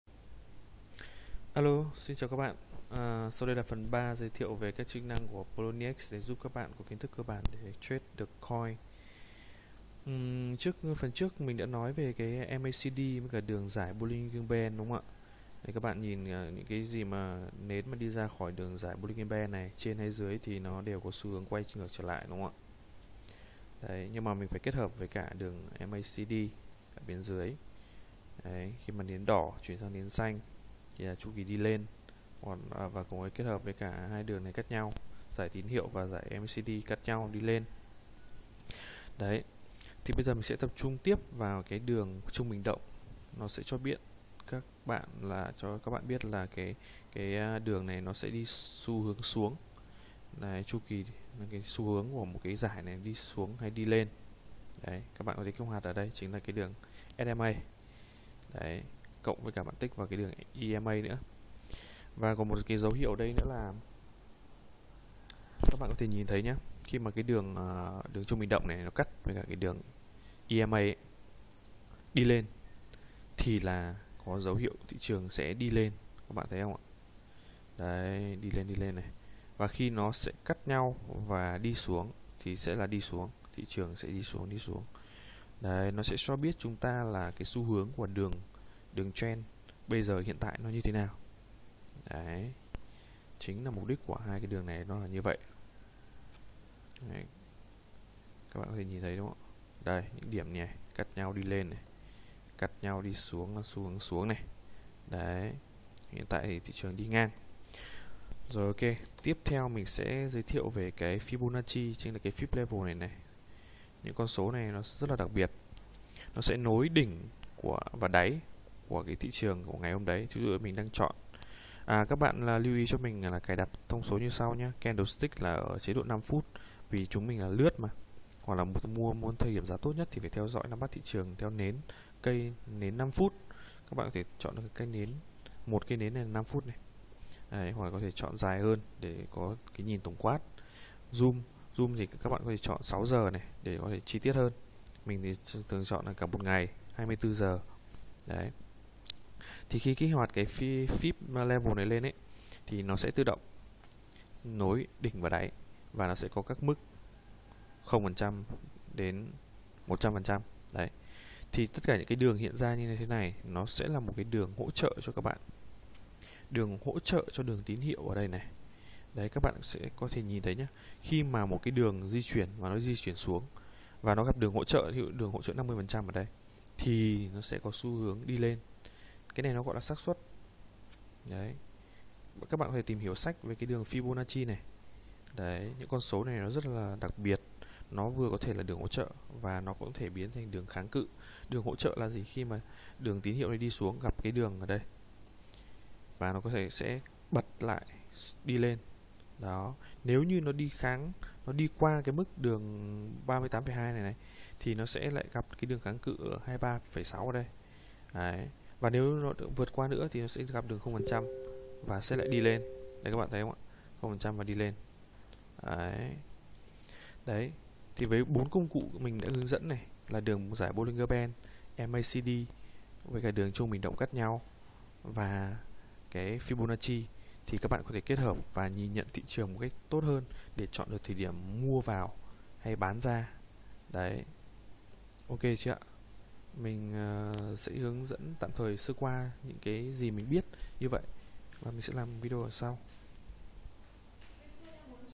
#3 Hướng dẫn cơ bản trade coin trên Poloniex (Phần 3)
Phần này mình giới thiệu cách nhìn: (mình đính chính lại là sử dụng nến 30 phút trở lên sẽ hiệu quả hơn nhé)
- Fib Levels
- SMA và EMA
Trade coin, hay còn gọi lướt sóng, là hình thức mua bán coin kiếm lời. Ví dụ giá nó xuống thì mình mua vào, giá lên lại bán ra, áp dụng với những coin dao động liên tục trong thời gian ngắn, có thể là trong 1-2 ngày, có khi nó dao động liên tục theo từng phút. Nói mua bán thì có vẻ thấy dễ quá, nhưng muốn làm giàu với Trade coin, bạn phải có kinh nghiệm rất nhiều. Trong đó có khả năng đọc hiểu được biểu đồ để biết được điểm dừng của giá, khả năng phán đoán và tính quyết đoán kèm theo yếu tố may mắn nữa. Đa số các coin đều bị chi phối bởi Bitcoin, nên để nắm được giá của nó bạn cũng phải nắm được tình hình Bitcoin.

Khi trade thì bạn chỉ nên trade những coin đã được niêm yết, tức là nó đã được cộng đồng chấp nhận giao dịch mua bán. Và thường các sàn giao dịch sẽ cung cấp những coin đã được phép giao dịch, những coin hợp lệ là những coin được niêm yết trên